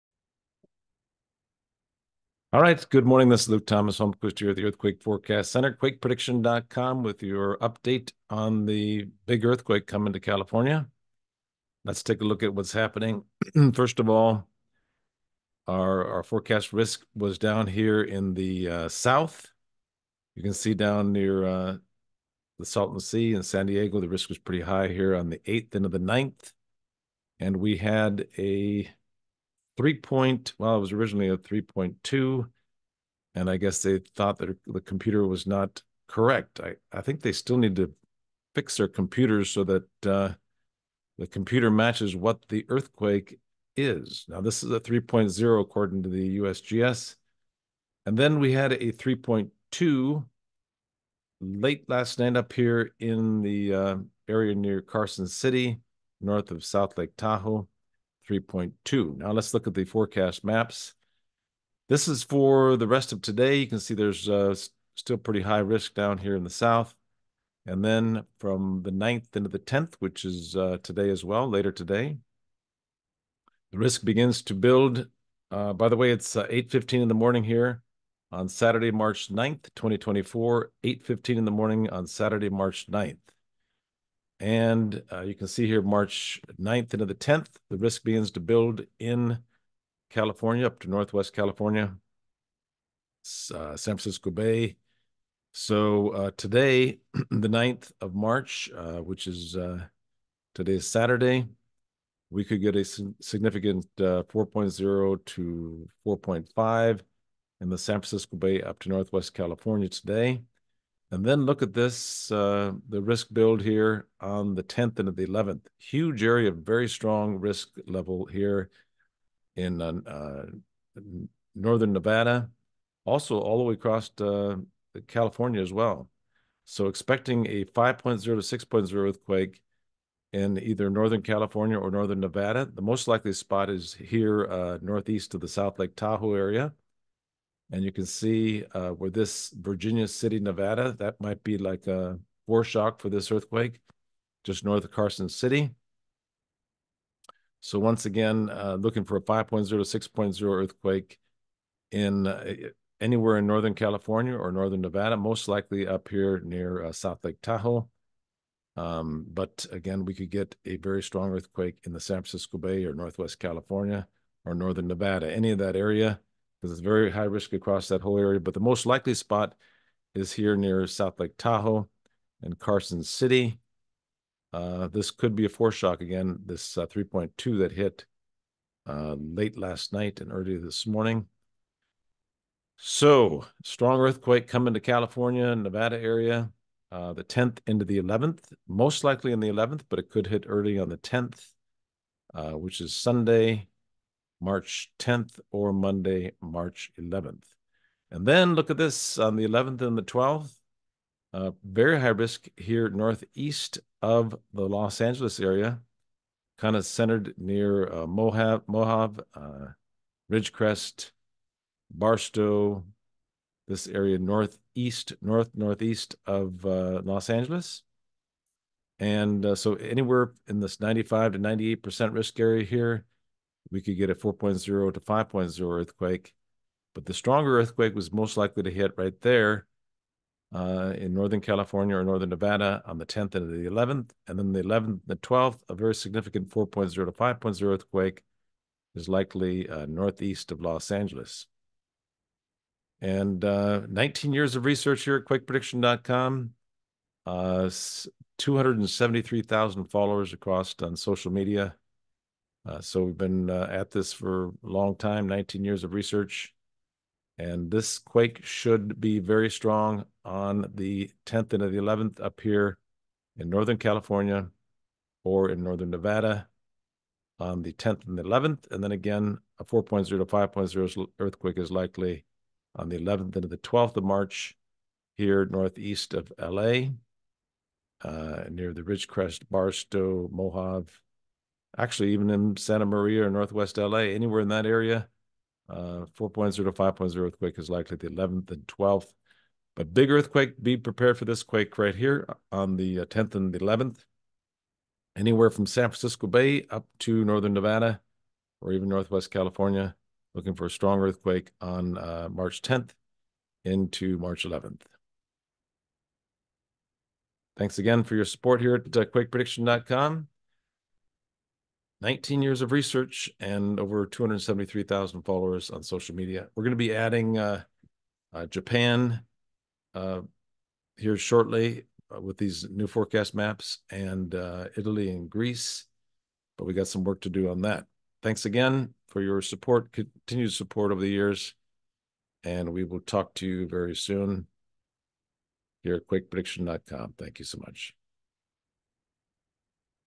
Strong earthquake is likely March 10 or March 11 in the Northern California to North Nevada area.
Strong earthquake is likely March 10 or March 11 in the Northern California to North Nevada area - please see forecast map